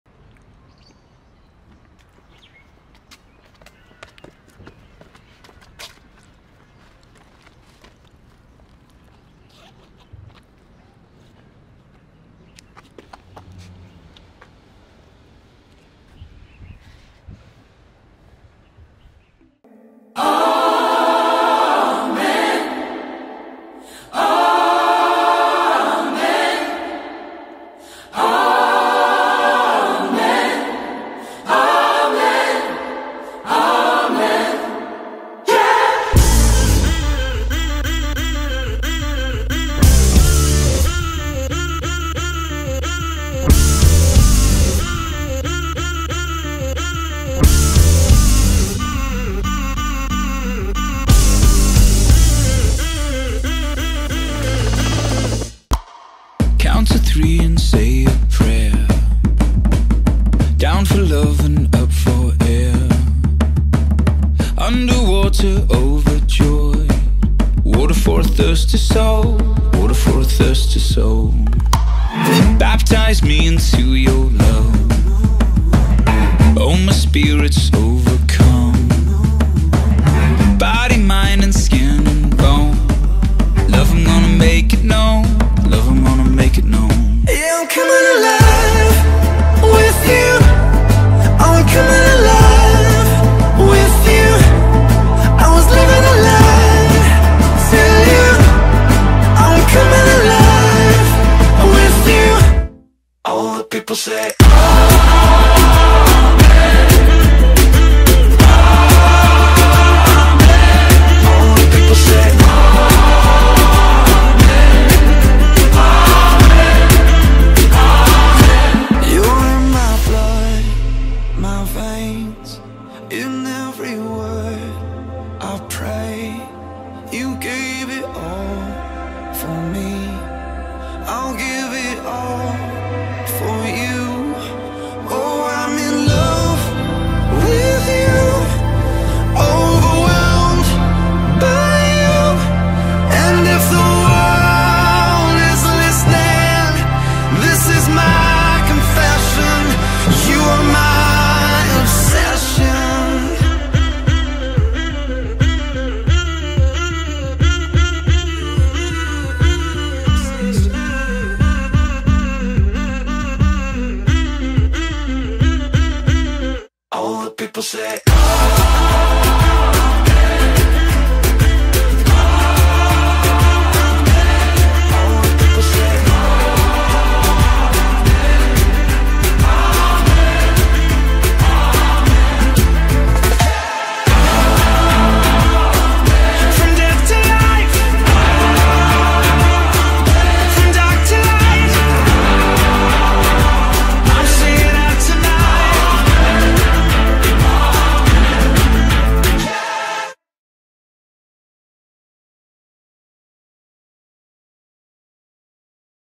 JoshJasper 195 - AMEN (feat. Brianna Chapman)
Amen is dance piece where i share my journey of transition from Breaking to Contemporary Dance. Both i and Brianna wanted to challenge our bodies to move in a manner that was out of our comfort zone and it surely worked. Special thanks to Clay Dance company where we both belong and to Watoto Church Ntinda for allowing to use the premises.
shot by One Shot Visuals.